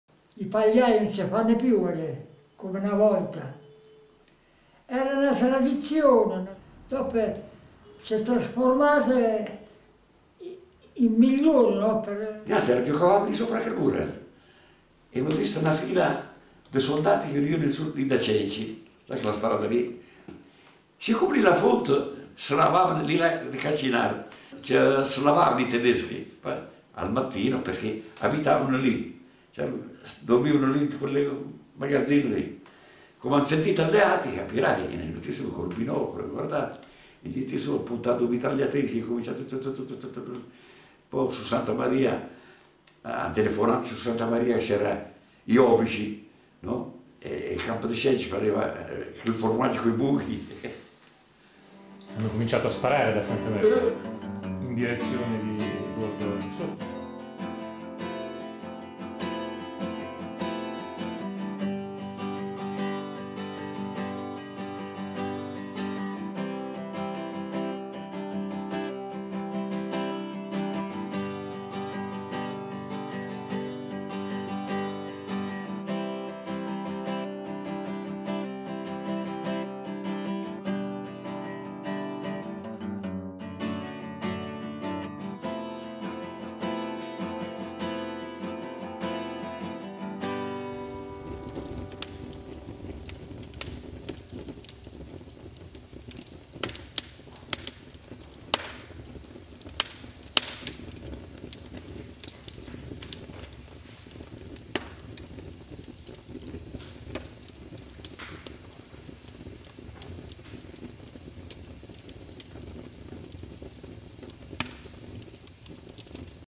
Intorno al fuoco
Giovanni, Walter, Mauro, Livia, Bruno, Elena e gli altri sono i protagonisti di questo breve filmato di presentazione del progetto Memoteca.